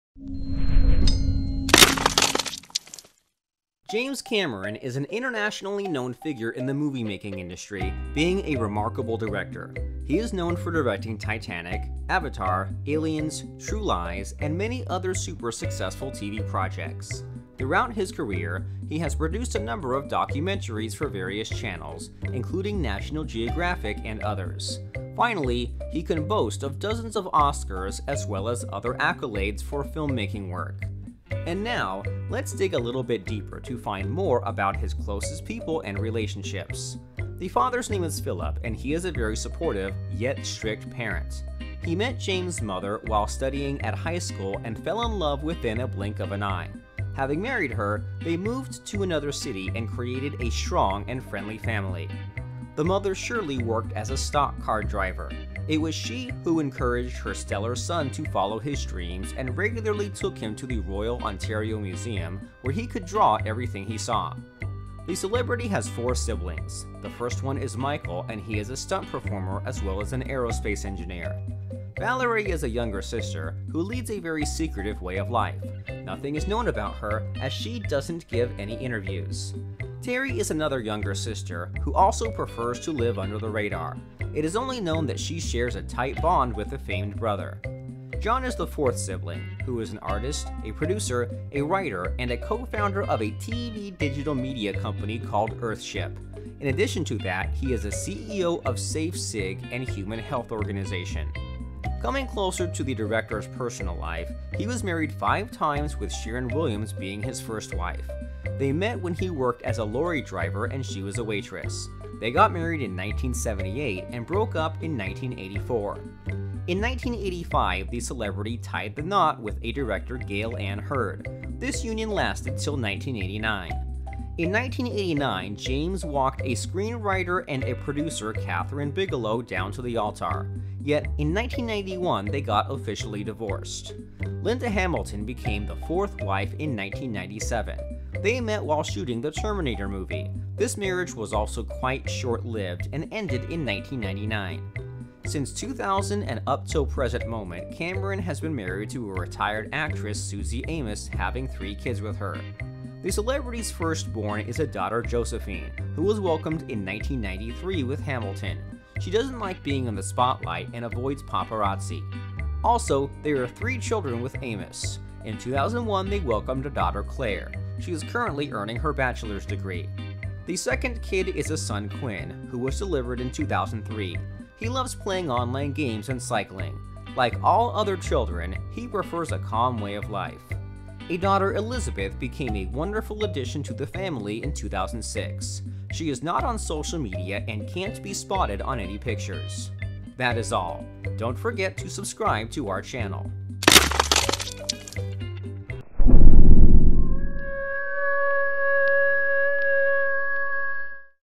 James Cameron Family (Wife, Kids, Siblings, Parents)
James Cameron is an internationally known figure in the moviemaking industry, being a remarkable director. He is known for directing Titanic, Avatar, Aliens, True Lies and many other super successful TV projects. Throughout his career, he has produced a number of documentaries for various channels, including National Geographic and others. Finally, he can boast of dozens of Oscars as well as other accolades for filmmaking work.
And now, let’s dig a little a bit deeper to find more about his closest people and relationships.

The father’s name is Philip and he is a very supportive, yet strict parent. He met James’ mother while studying at high school and fell in love within a blink of an eye. Having married her, they moved to another city and created a strong and friendly family.

The mother Shirley worked as a stock car driver. It was she, who encouraged her stellar son to follow his dreams and regularly took him to the Royal Ontario Museum, where he could draw everything he saw.

The celebrity has four siblings. The first one is Michael and he is a stunt performer as well as an aerospace engineer.

Valerie is a younger sister, who leads a very secretive way of life. Nothing is known about her, as she doesn’t give any interviews.

Terri is another younger sister, who also prefers to live under the radar. It is only known that she shares a tight bond with a famed brother.

John is the fourth sibling, who is an artist, a producer, a writer and a co-founder of a TV digital media company called Earthship. In addition to that he is a CEO of SafeCig and Human Health Organization.

Coming closer to the director’s personal life, he was married five times with Sharon Williams being his first wife. They met when he worked as a lorry driver and she was a waitress. They got married in 1978 and broke up in 1984.

In 1985 the celebrity tied the knot with a director Gale Anne Hurd. This union lasted till 1989.

In 1989 James walked a screenwriter and a producer Kathryn Bigelow down to the altar. Yet, in 1991 they got officially divorced.

Linda Hamilton became the fourth wife in 1997. They met while shooting the Terminator movie. This marriage was also quite short-lived and ended in 1999.

Since 2000 and up till present moment Cameron has been married to a retired actress Suzy Amis having three kids with her.

The celebrity’s firstborn is a daughter Josephine, who was welcomed in 1993 with Hamilton. She doesn’t like being in the spotlight and avoids paparazzi.

Also, there are three children with Amis. In 2001 they welcomed a daughter Claire. She is currently earning her bachelor’s degree.

The second kid is a son Quinn, who was delivered in 2003. He loves playing online games and cycling. Like all other children, he prefers a calm way of life.

A daughter Elizabeth became a wonderful addition to the family in 2006. She is not on social media and can’t be spotted on any pictures.